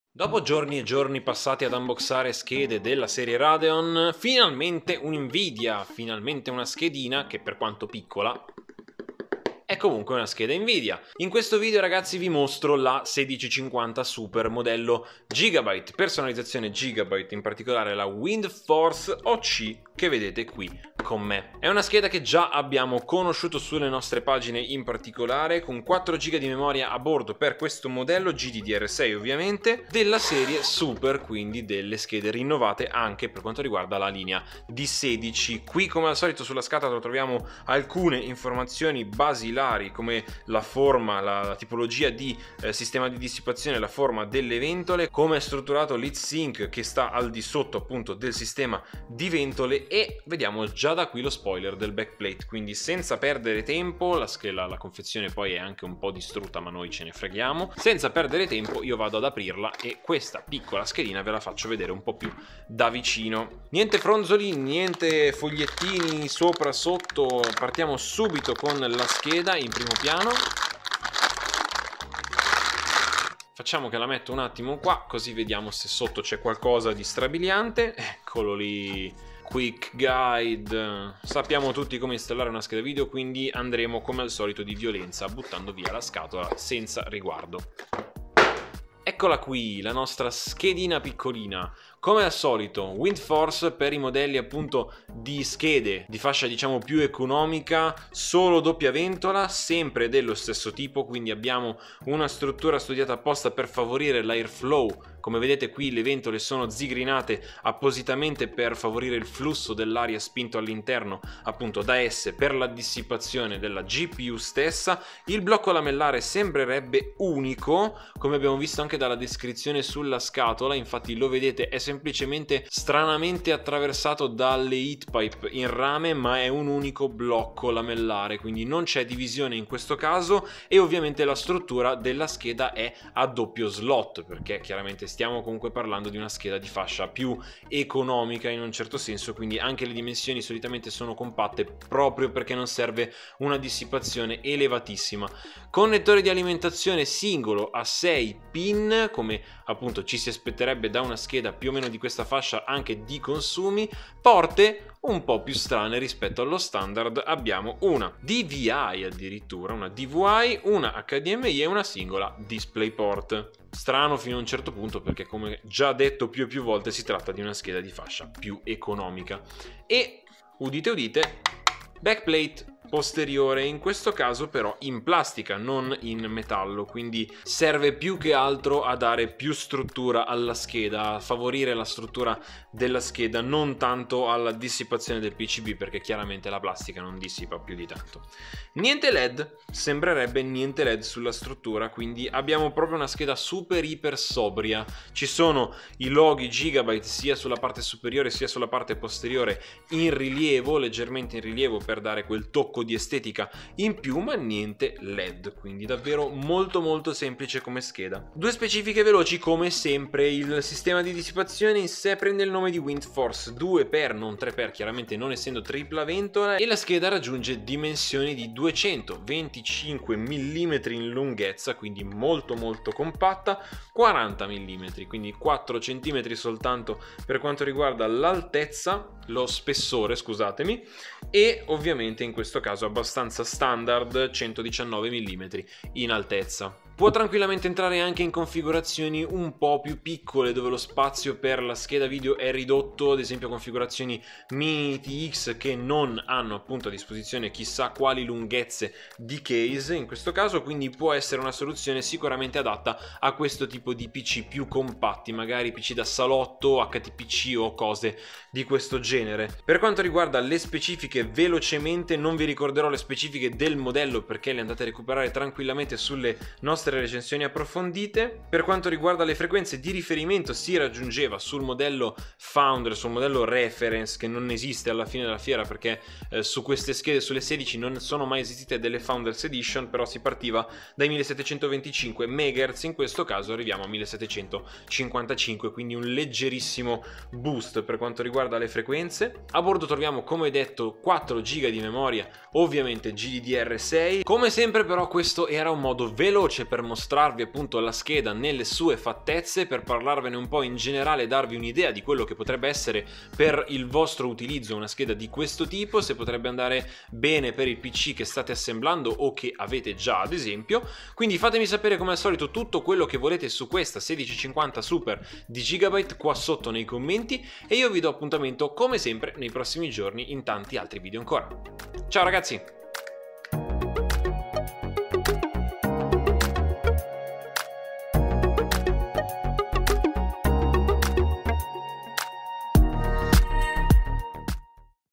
GIGABYTE GTX 1650 SUPER WINDFORCE OC 4G | UNBOXING
Nuovo giorno, nuova scheda da conoscere! Oggi ho qui con me la GTX 1650 Super in versione Gigabyte Windforce OC 4G, personalizzazione doppia ventola davvero interessante. Unboxing time!

● Gigabyte GTX 1650 Super su Amazon

▌ OFFERTE E SCONTI
Sconti Amazon

▌ ATTREZZATURA VIDEO